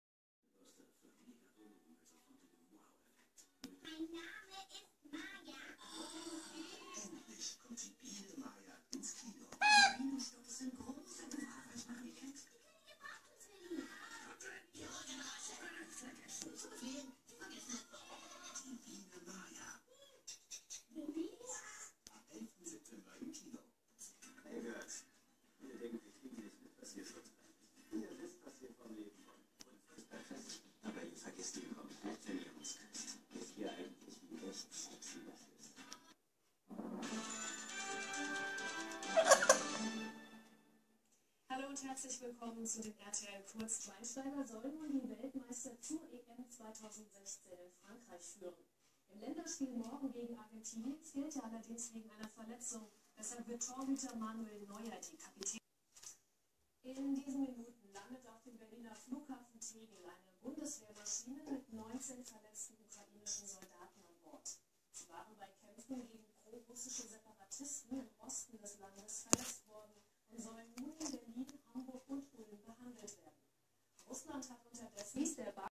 NICO DER ATTENTÄTER u. MARIE. .anstarren
Graupapagei Nico und Amazone Marie starren sich wieder an...